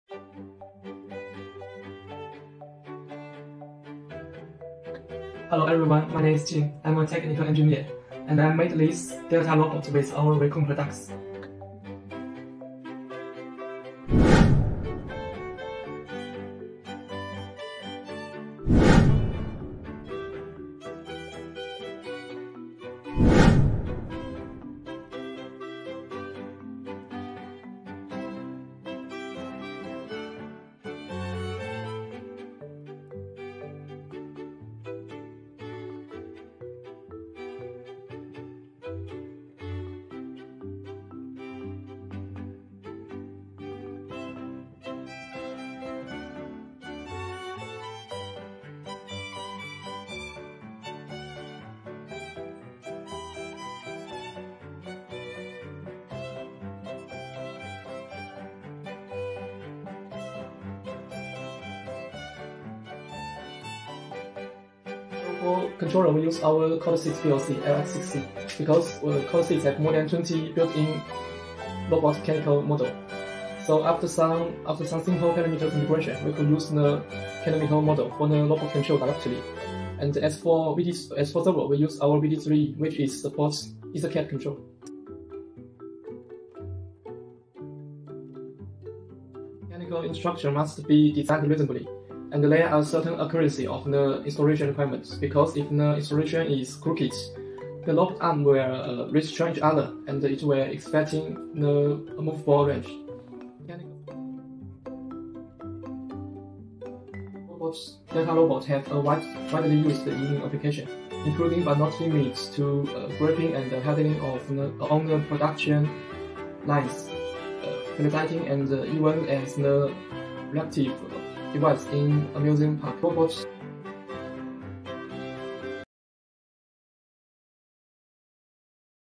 Parallel Robot Solution with Wecon LX6C PLC and VD3E Servo
LX6C PLC: 🔹Built-in multiple motion models.
                    🔹Simple configuration of parameters.
                    🔹Supports a variety of coordinate systems;
VD3E  Servo: 🔹Real Time
                        🔹High Precision
                        🔹Easy Wiring
                        🔹Anti-interference